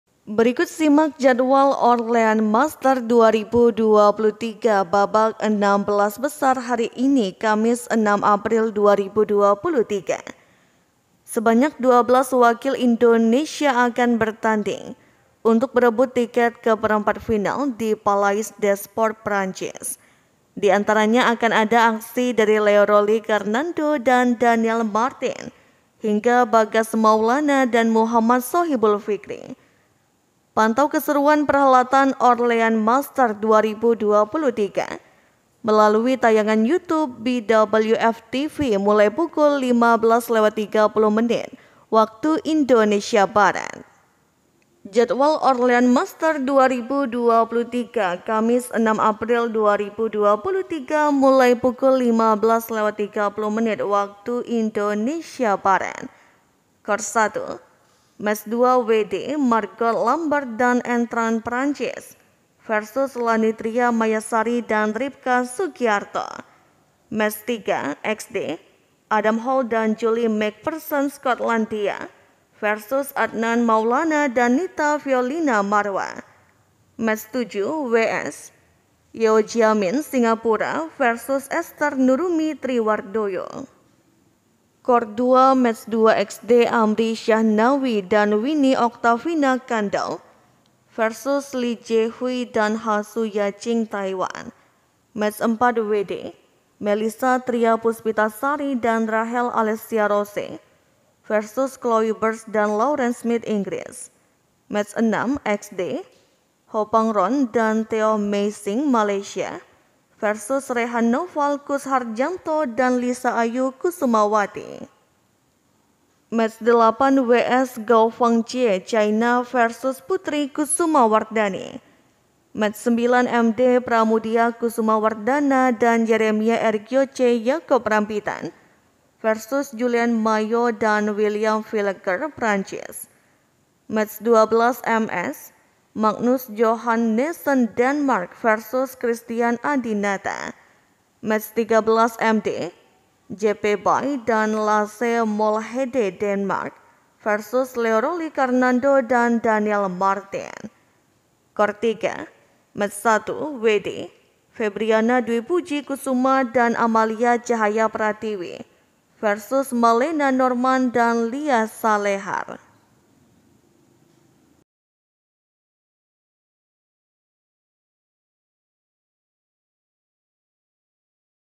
🔴 Jadwal Orleans Masters 2023 Hari Ini: 12 Wakil Indonesia Bertanding, Tonton di Youtube BWF TV
Berikut simak jadwal Orleans Masters 2023 babak 16 besar hari ini Kamis (6/4/2023).

Sebanyak 12 wakil Indonesia akan bertanding untuk berebut tiket ke perempat final di Palais des Sports, Prancis.

Di antaranya akan ada aksi dari Leo Rolly Carnando/Daniel Mathin hingga Bagas Maulana/Muhammad Shohibul Fikri.

Pantau keseruan perhelatan Orleans Masters 2023 melalui tayangan Youtube BWF TV mulai pukul 15.30 WIB.

Jadwal Orleans Masters 2023, Kamis (6/4/2023):

Mulai pukul 15.30 WIB

Court 1

Match 2 [WD] - Margot Lambert/Anne Tran (Prancis) vs Lanny Tria Mayasari/Ribka Sugiarto

Match 3 [XD}] - Adam Hall/Julie Macpherson (Skotlandia) vs Adnan Maulana/Nita Violina Marwah

Advertisement by
Match 7 [WS] - Yeo Jia Min (Singapura) vs Ester Nurumi Tri Wardoyo

Court 2

Match 2 [XD] - Amri Syahnawi/Winny Oktavina Kandow vs Lee Jhe-Huei/Hsu Ya Ching (Taiwan)

Match 4 [WD] - Melysa Tria Puspita Sari/Rachel Allessya Rose vs Chloe Birch/Lauren Smith (Inggris)

Match 6 [XD] - Hoo Pang Ron/Teoh Mei Xing (Malaysia) vs Rehan Naufal Kusharjanto/Lisa Ayu Kusumawati

Match 8 [WS] - Gao Fang Jie (China) vs Putri Kusuma Wardani

Match 9 [MD] - Pramudya Kusumawardana/Yeremia Erich Yoche Yaboc Rambitan vs Julien Maio/William Vilegger (Prancis)

Match 12 [MS] - Magnus Johannesen (Denmark) vs Christian Adinata

Match 13 [MD] - Jeppe Bay/Lasse Molhede (Demark) vs Leo Rolly Carnando/Daiel Marthin

Court 3

Match 1 [WD] - Febriana Dwipuji Kusuma/Amalia Cahaya Pratiwi vs Malena Norrman/Lia Salehar